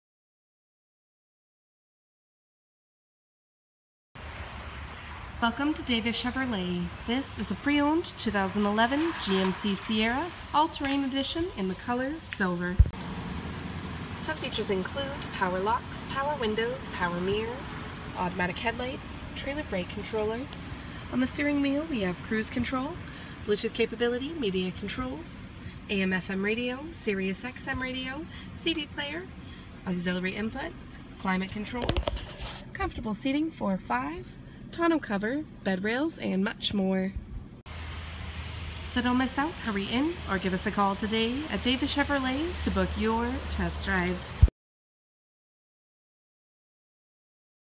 2011 GMC Sierra 1500 SLE | Davis Chevrolet | Airdrie AB
Looking for the perfect pre-owned truck?? Look no further thant his 2011 GMC Sierra with great features such as remote start/entry, trailer brake controller, All-Terrain package, USB port, power adjustable pedals, comfortable seating for 5, crew cab, short box, cruise control, bluetooth, CD player and more!!

114969